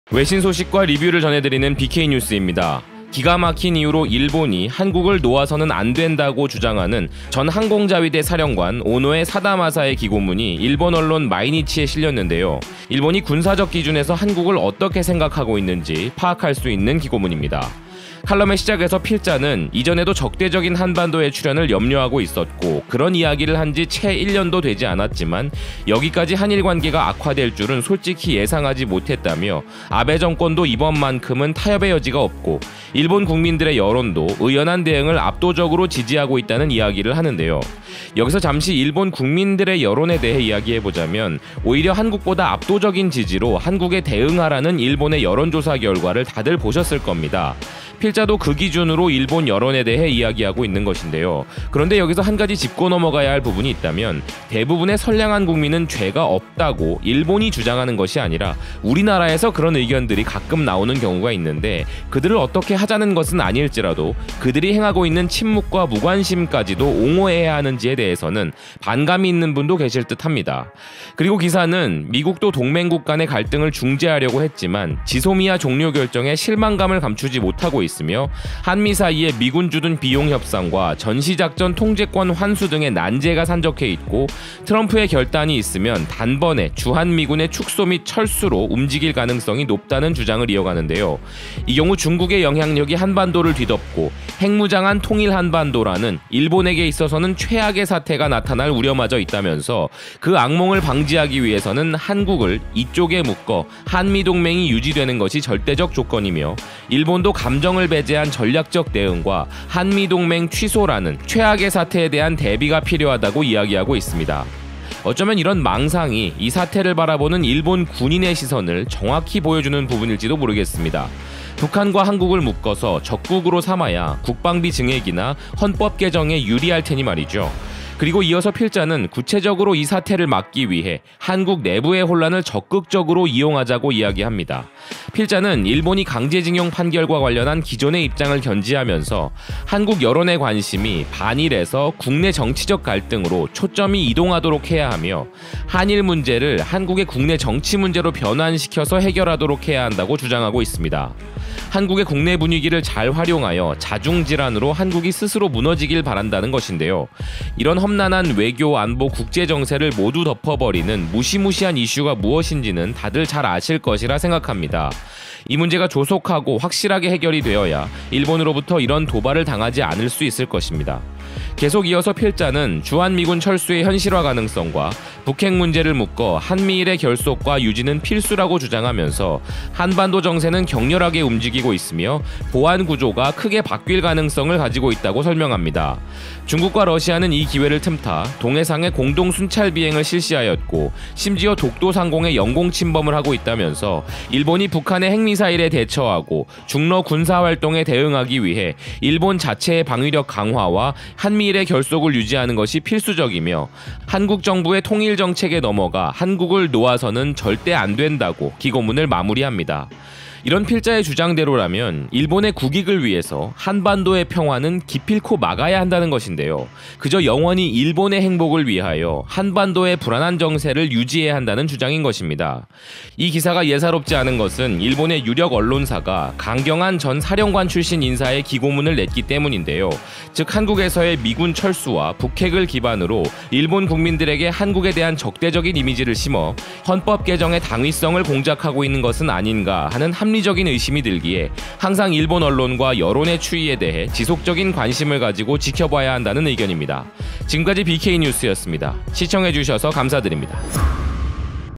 한국을 놓아 주면 일본에게 악몽이 된다는 전 자위대사령관 "한국의 탈출을 기필코 막아야 한다."
아무리 국내적으로 혼란스러워도
우리의 관심은 이어져야 합니다.

공유도 많이 부탁 드려요~!